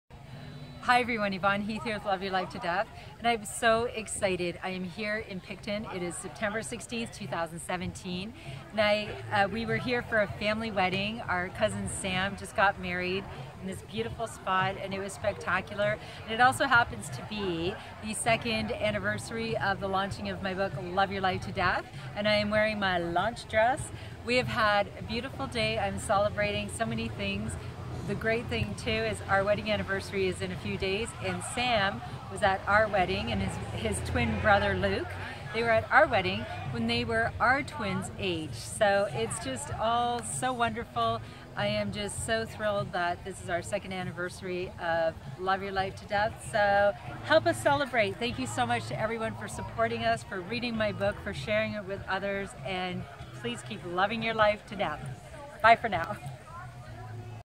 So Much to Celebrate!
Hi everyone! We are here in Picton, ON and it is September 16th, 2017. We are here for a family wedding; Cousin Sam just married his lovely bride Jessica. It is a beautiful place and a spectacular day!